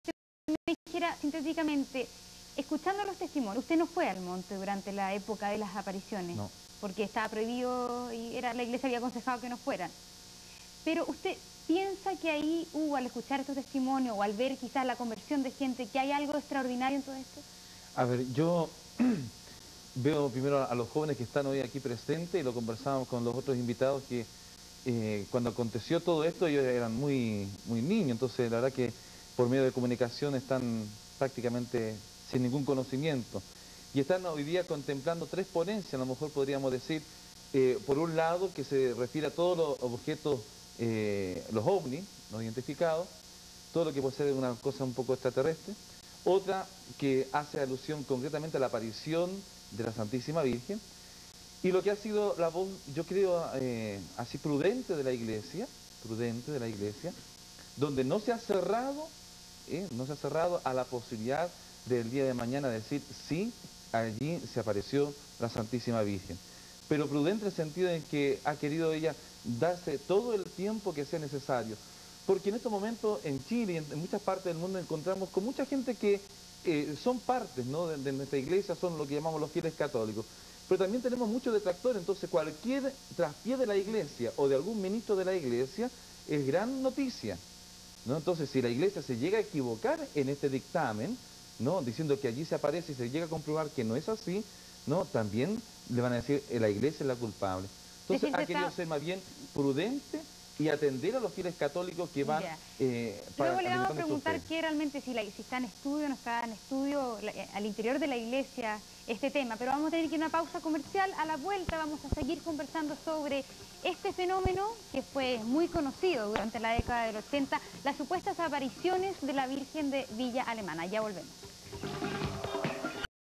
Apariciones de Peñablanca - Testimonio del Padre Álvaro Bernal
Testimonio del Padre Álvaro Bernal ... Cree que hubo algo extraordinario ahí

El Padre Álvaro Bernal nació el 11 de junio del año 1962. Fue ordenado sacerdote el 20 de noviembre del año 1988 por el entonces obispo de Valparaíso monseñor. Francisco de Borja Valenzuela.

Ofició su primera misa en la Fiesta de Cristo Rey. Sus estudios básicos los efectuó en el Colegio de las Religiosas de la Sagrada Familia de Urgel "Ana María Janer" en Miraflores, y la enseñanza media fueron en el Colegio Seminario San Rafael de Viña del Mar.

Los estudios sacerdotales los efectuó en el Pontificio Seminario Mayor San Rafael de Lo Vásquez.

Como sacerdote su primer destino fue la Parroquia Santo Nombre de Jesús de La Calera. Fue párroco de tres parroquias de nuestra Diócesis: Parroquia "Nuestra Señora de Andacollo" del Cerro Ramaditas de Valparaíso; párroco fundador del nuevo Templo de la Parroquia "Sagrado Corazón de Jesús", situada en el sector de Valencia de Quilpué y párroco de "Los Doce Apóstoles", ubicada a un costado del Congreso Nacional. También fue Delegado Episcopal para el Diaconado Permanente.

Desde el 11 de marzo era Vicario General del Obispado de Valparaíso y Moderador de la Curia Diocesana.

El padre Alvaro Bernal fue el fundador de la Parroquia del Sagrado Corazón de Jesús de Quilpué en1998. Ejerció hasta marzo del 2005.(Q.E.P:D)